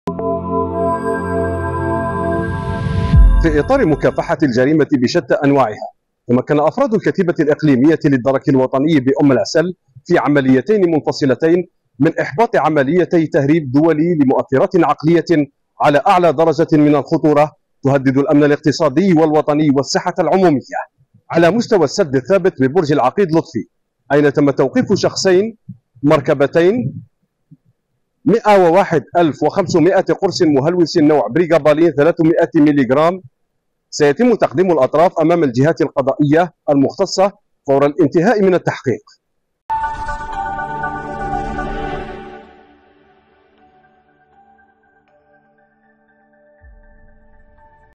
تندوف : الدرك الوطني يحجز أزيد من 100 ألف قرص مهلوس
تندوف (وأج) - تمكنت مصالح الدرك الوطني من احباط عملية تهريب دولية تمكنت على إثرها من حجز أكثر من  100 ألف قرص مهلوس، كما تم توقيف شخصين سيتم تقديمهما أمام الجهات القضائية المختصة فور الانتهاء من التحقيق.